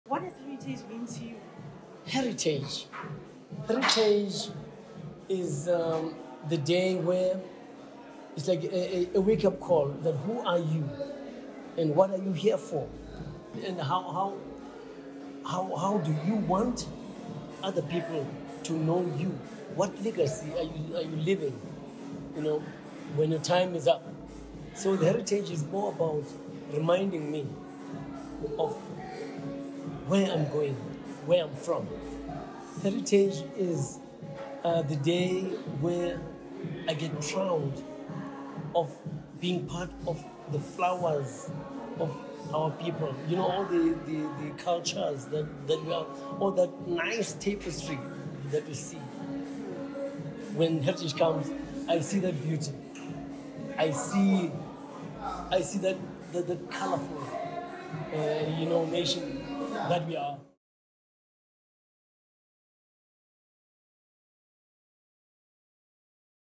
What does Heritage Month mean to Ringo Madlingozi
Apple Music has launched the Amaqhawe campaign for Heritage Month, featuring six exclusive guest-curated playlists by some of the country’s most iconic artists such as Ringo Madlingozi, and this is what Heritage month means to Ringo.
Video: Carlos Muchave